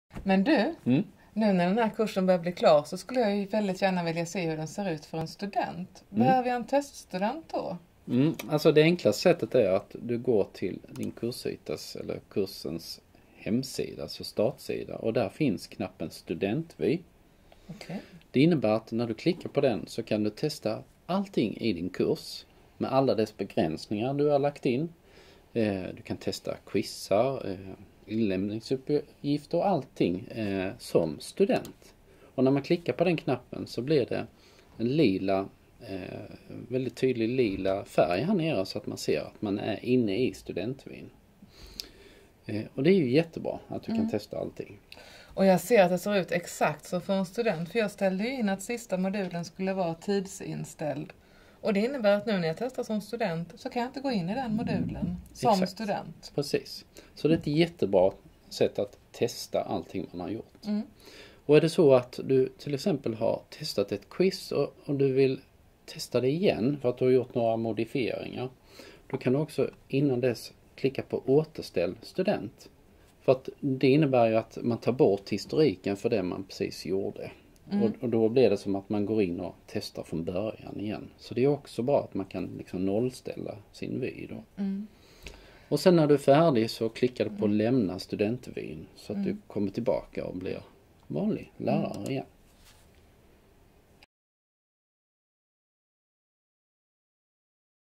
Studentvy i Canvas
Hur du ser din kurs som student

Producerad av Åsa Kronkvist och Dan Wirdefalk, LärandeResursCentrum, Högskolan Kristianstad